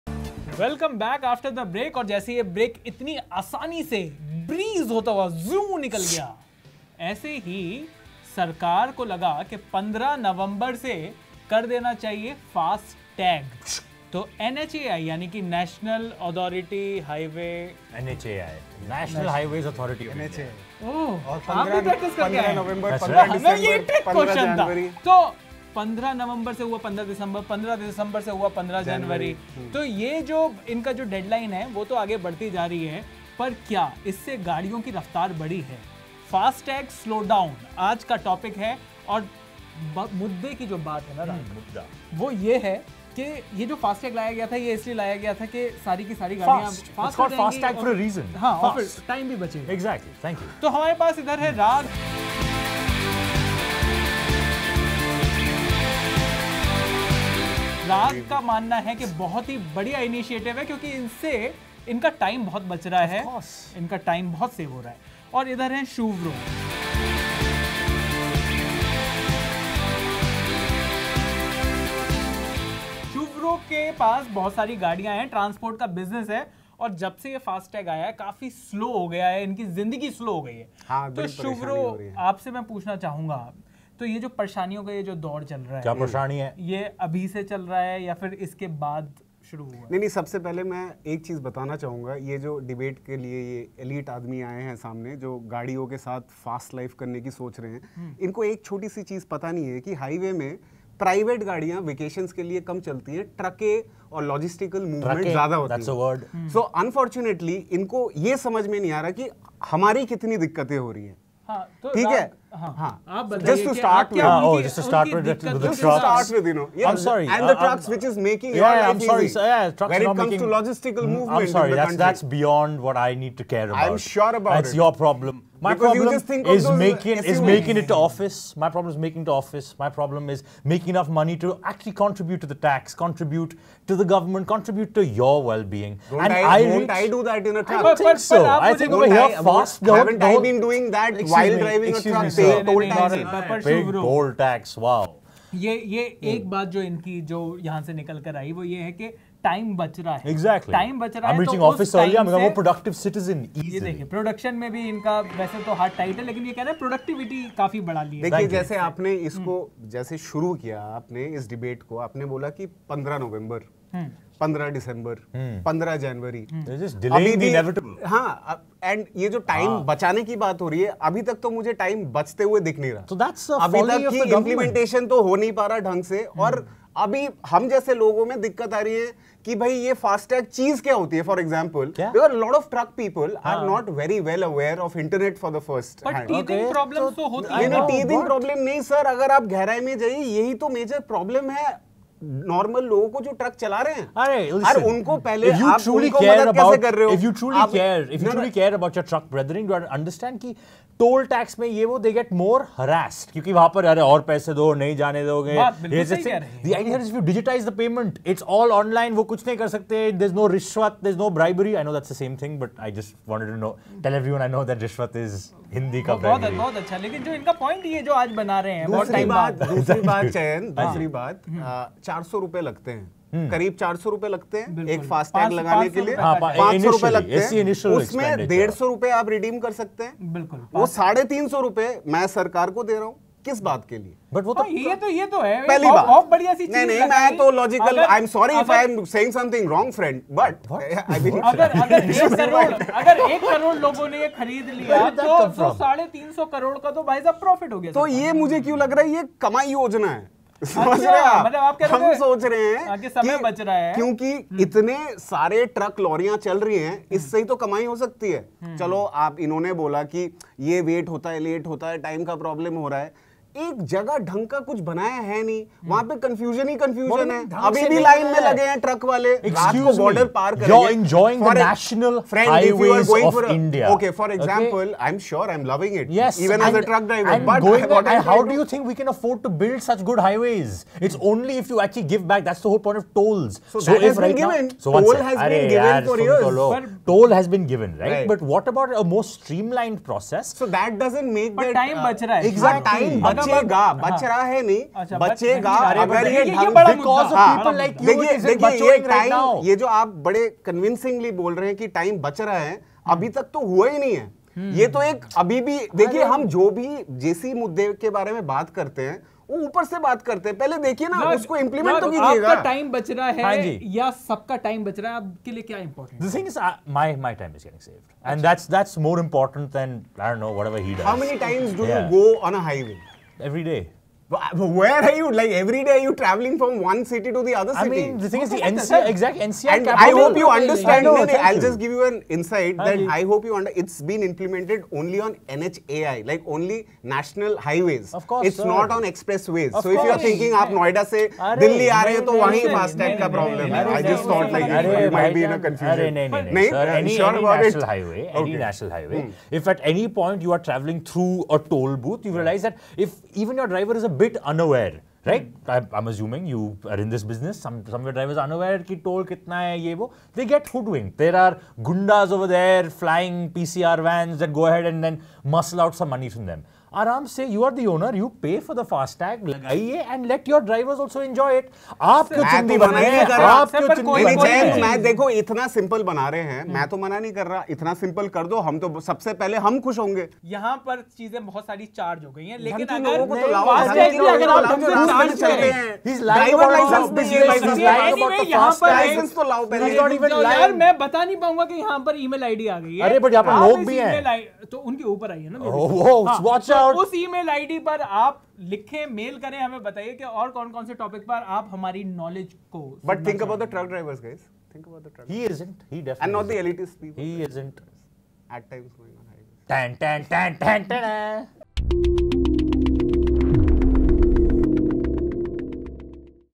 Are FASTags Making Life Slow?
Are FASTags actually making things fast or are we approaching a FASTag slowdown? Let's find out what the (crazy) debaters have to say!